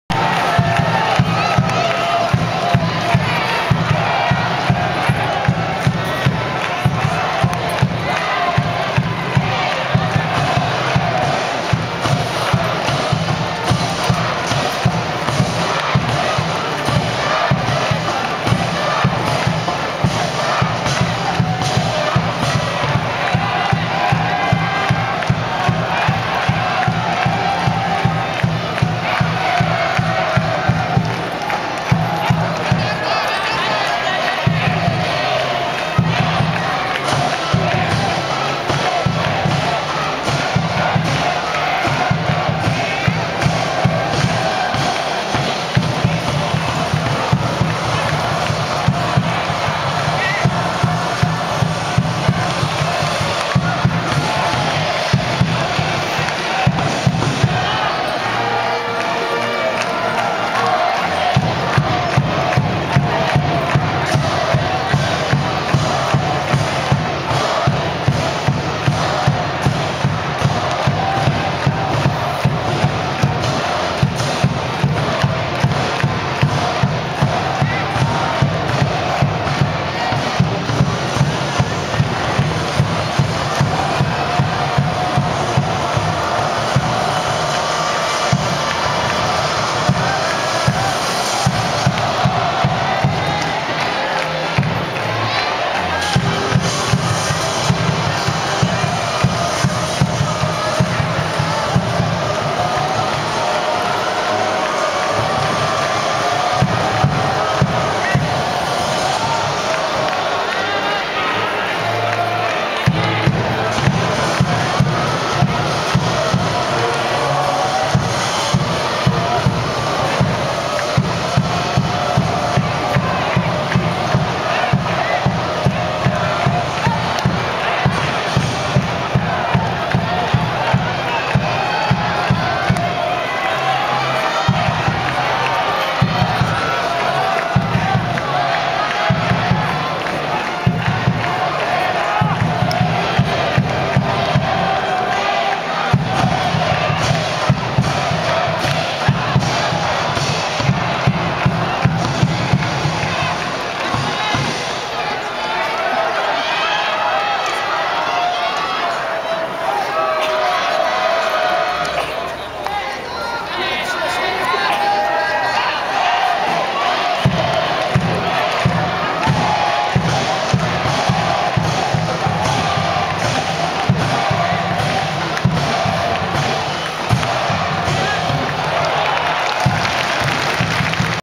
東大 鈴木 対 法大 伊藤諒_2011100802
2011年秋季リーグ 東大 対 法大 1回戦の東大 鈴木 対 法大 伊藤諒です。フルカウントから・・・。